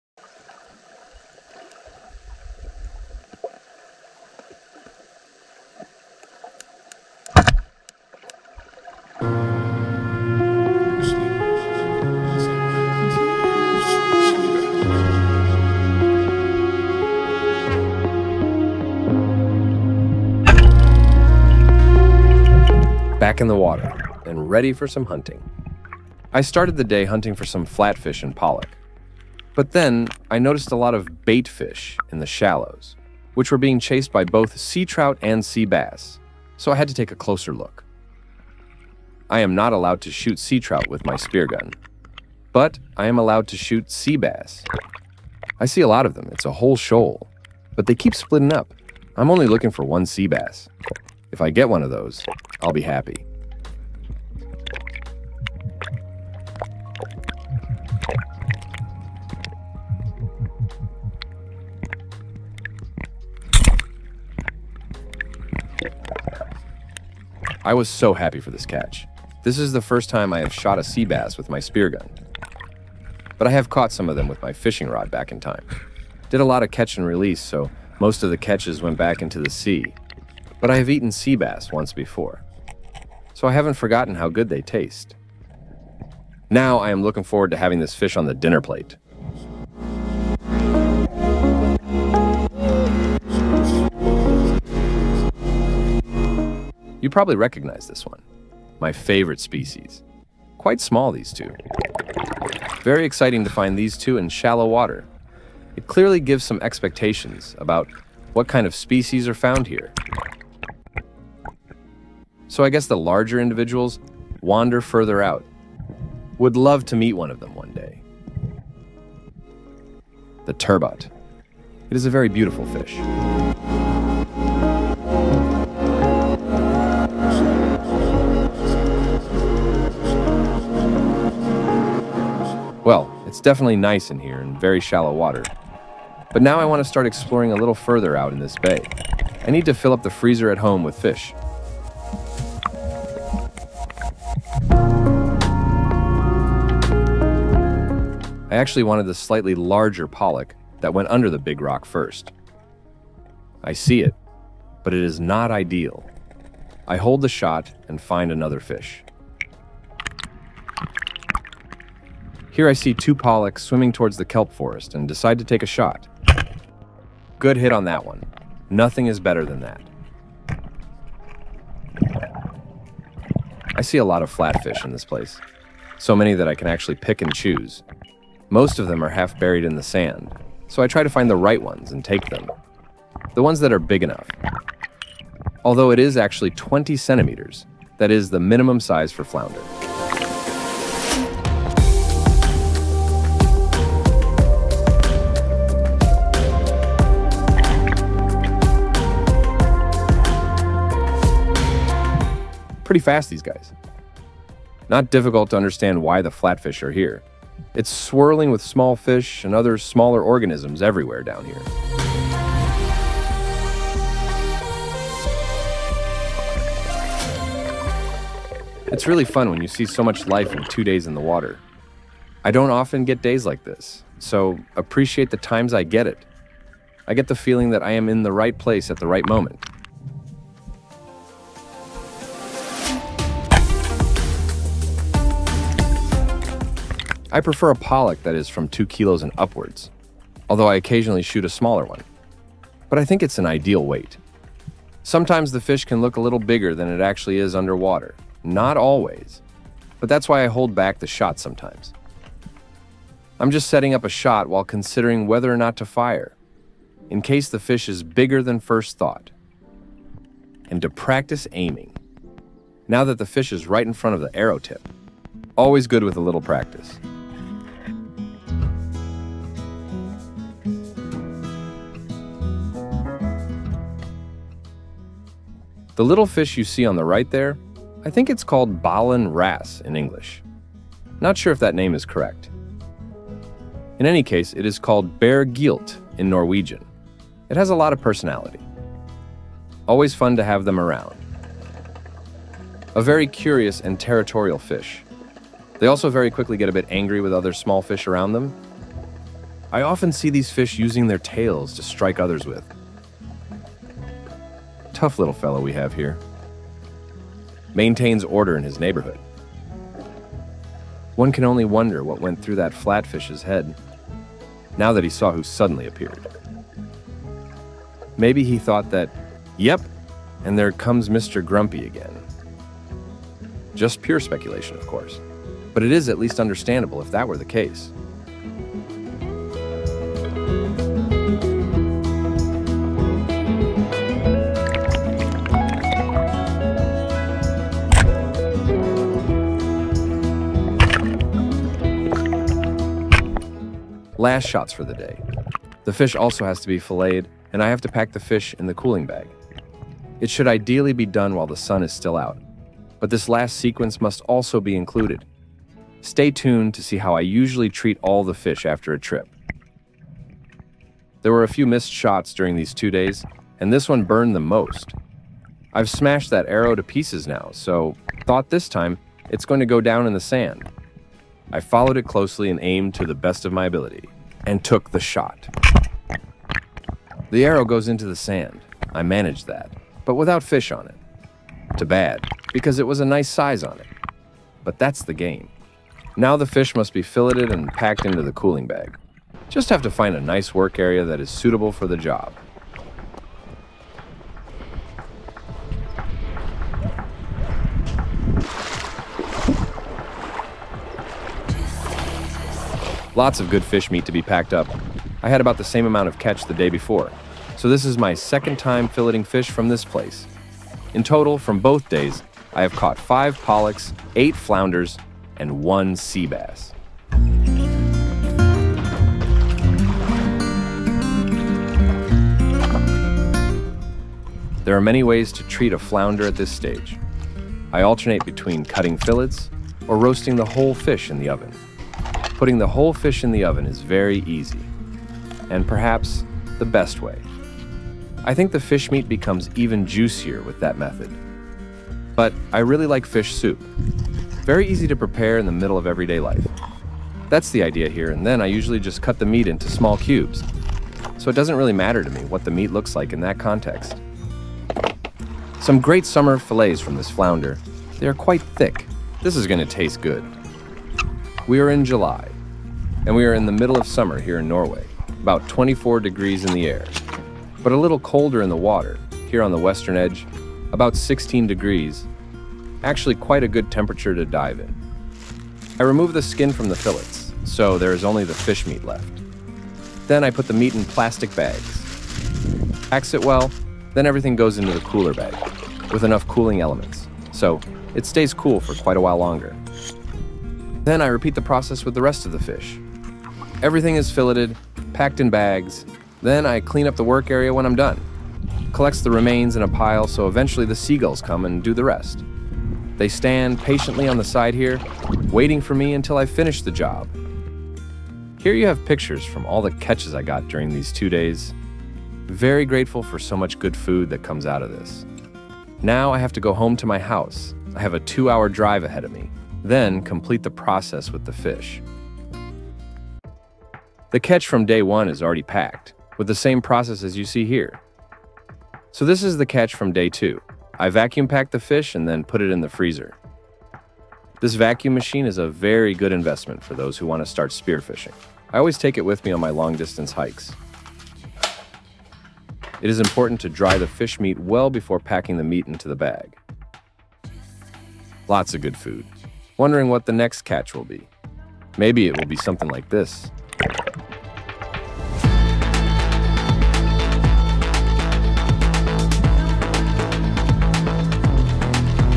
Spearfishing - Flatties, Bass&Pollock And Meet Mr. Grumpy - Norway
Spearfishing in Norway - Grotlesanden (Bremanger)

During to days I caught 5 pollock, 8 flounder and one seabass. Got to experience the moment when life is buzzing underwater. Follow the journey in beautiful surroundings with me at Grotlesanden in Bremanger.